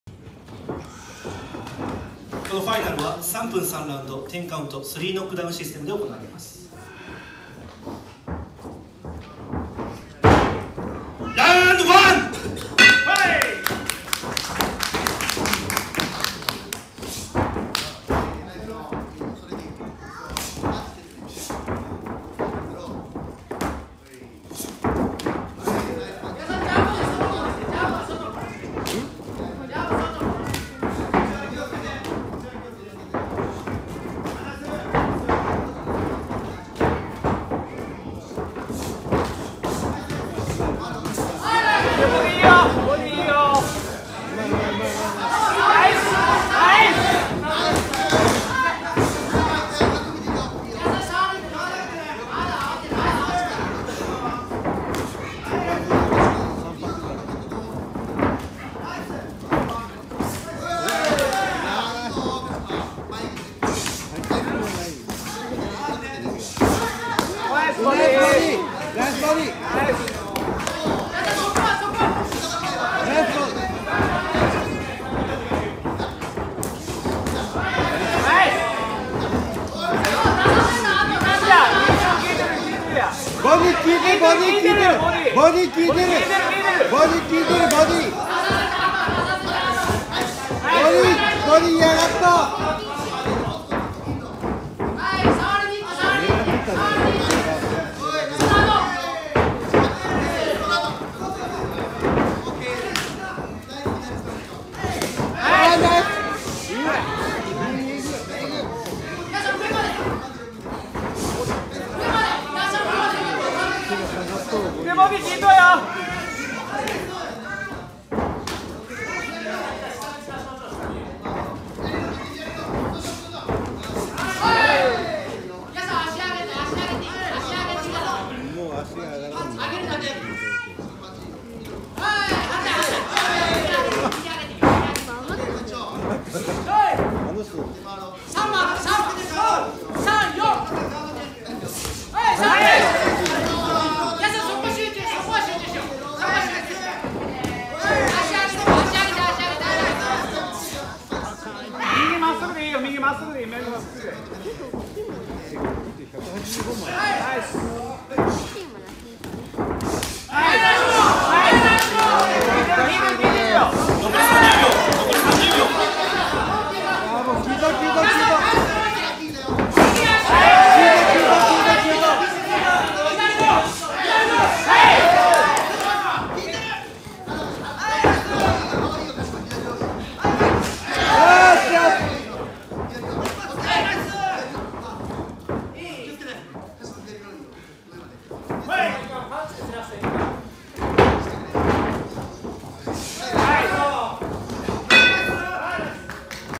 KINGEXCEED主催イベント「KING3」【児島昭男選手 VS 井手泰晴館長 (ROUND1)】
2019年3月に行われた福岡市城南区にあるキックボクシングジムKINGEXCEED主催のイベント「KING3」
リアルディール福岡所属のプロ選手児島昭男選手と、KINGEXCEED館長井手泰晴氏の試合の様子です（1ラウンド目）

ーーーー
福岡市城南区のキックボクシングジム「KING EXCEED」HP

ーーーー
リアルディールHP